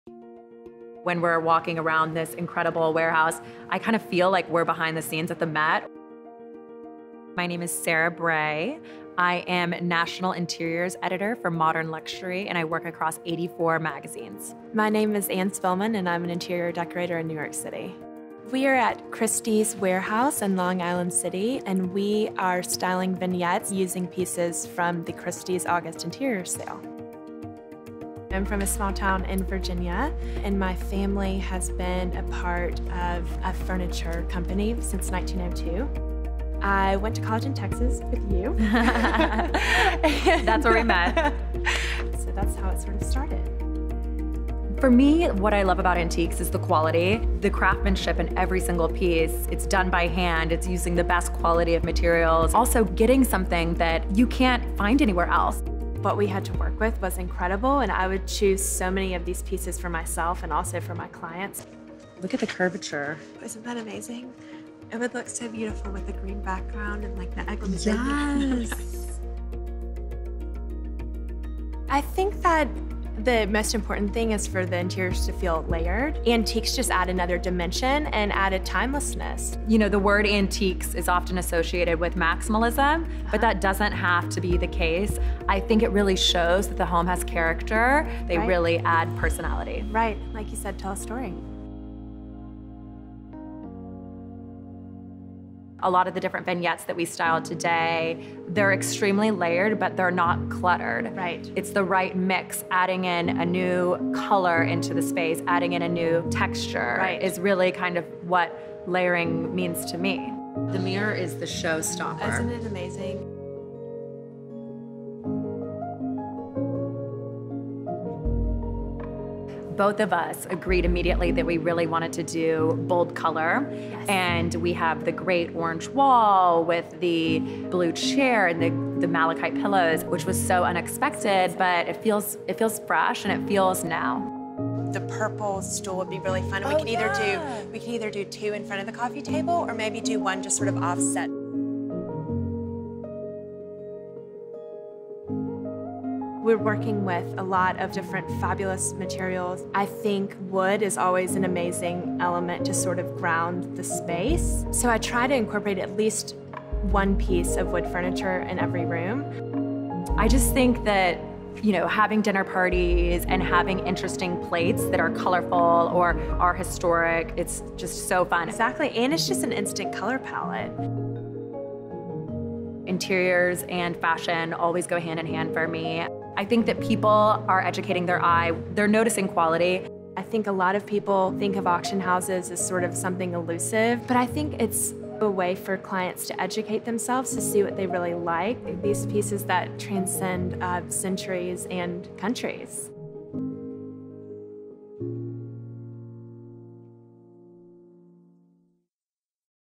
The Tastemakers: Sarah Bray and Anne Spilman | Christie's
Sarah Bray, interiors editor at Modern Luxury magazine, and interior designer Anne Spilman create a series of vignettes from luxurious objects offered in the August Interiors sale​.